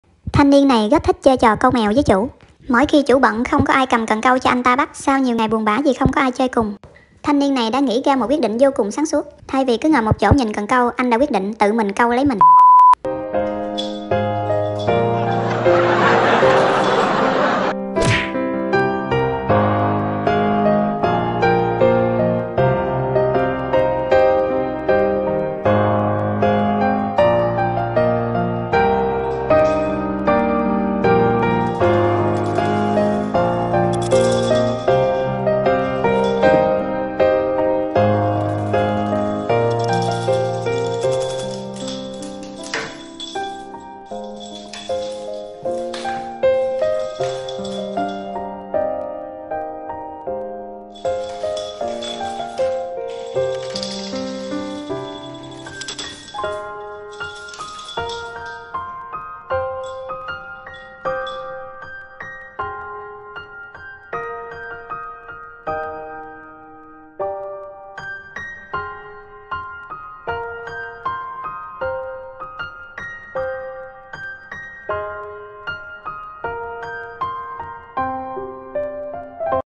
Khế thúi tự chơi một mình
ko còn Quách, khế tự chơi ên với cần câu mèo